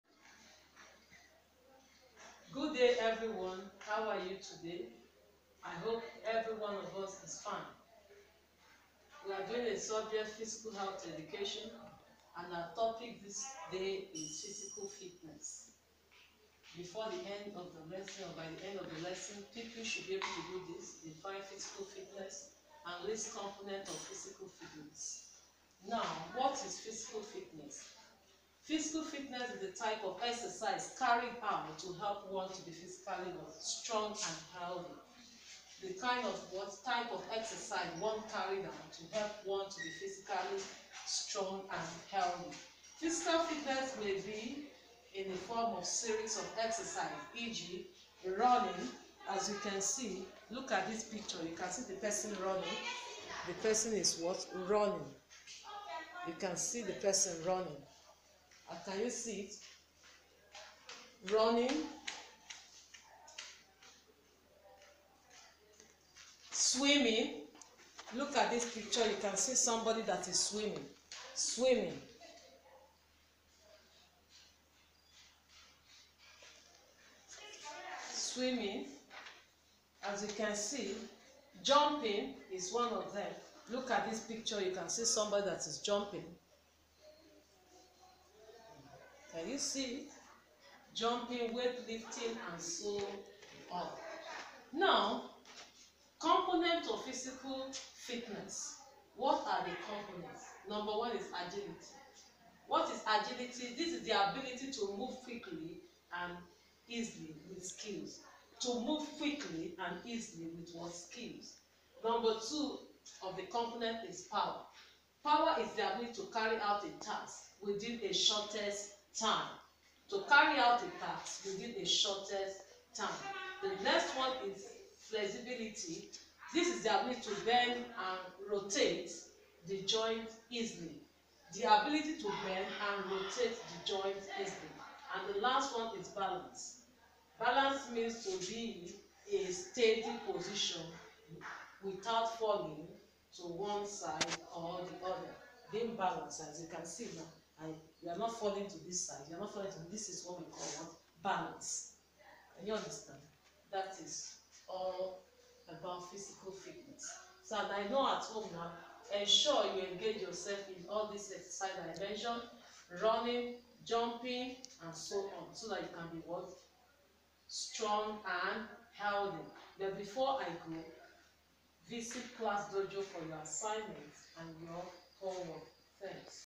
Physical and Health Education - Basic 6 (Week 1)
Learn to define physical fitness and list components of physical fitness.